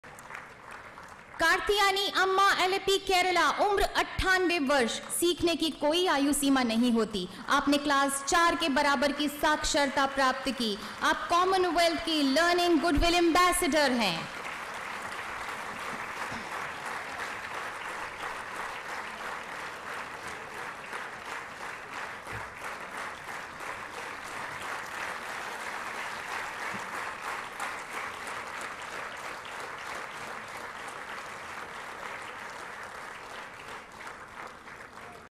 President Kovind presented the Nari Shakti Puraskar to Karthyayini Amma (98) from Alappuzha, Kerala.
President Kovind presented the Nari Shakti Puraskar to Karthyayini Amma (98) from Alappuzha, Kerala. Karthyayini Amma appeared for writing the fourth standard equivalency course under Kerala Literacy Mission’s Akasharalaksham Scheme in August 2018.  She bagged the first rank and scored 98% marks.  She aspires to clear tenth standard equivalent examination when she turns 100 and also acquire computer skills. She was also selected as the Commonwealth of Learning Goodwill Ambassador.